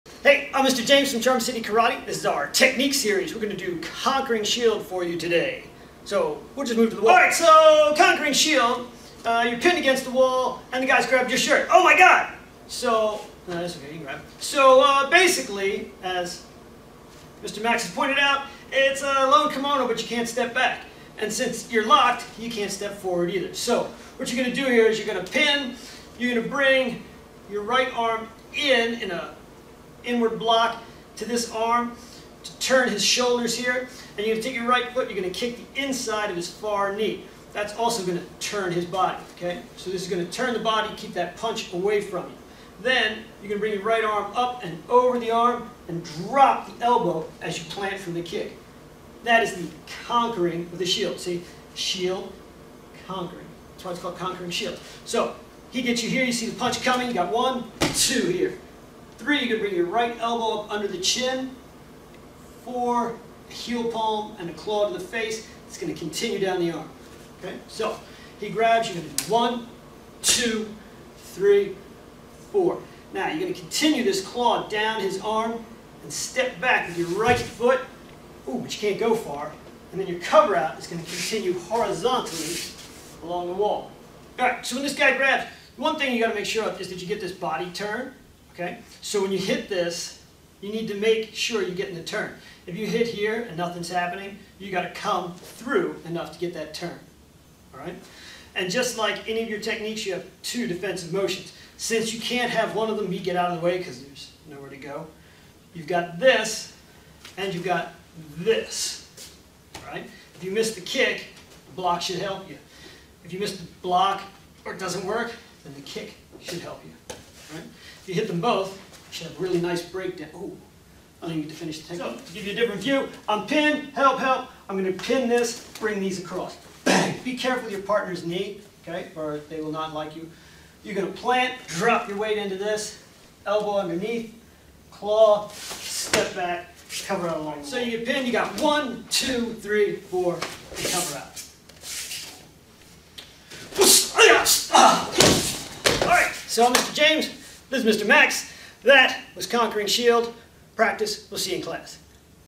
Conquering Shield - Kenpo self defense technique for a left grab and right punch
Self Defense Techniques: In this episode Mr James of Charm City demonstrates Conquering Shield, which is a Kenpo self defense technique for a left lapel grab with a right punch.

Thanks to Mr Max for taking the whoopings!

Conquering Shield (front left stiff arm lapel grab)
1. With feet together, simultaneously pin opponent's left arm with your left hand as your right vertical forearm strikes against opponent's left elbow to break and as your right ball of foot kicks to
inner knee cap of opponent's right leg.
2. As you proceed to plant your right foot forward (to 11:00) into a right neutral have your right arm glance up and then strike down with your right downward elbow to opponent's left forearm thus forcing your opponent's head down.
3. With feet firmly planted, deliver a right upward elbow under opponent's chin.
4. Follow up with a right downward heel and claw to opponent's face.
5. Right front crossover and cover out twice to 6:00.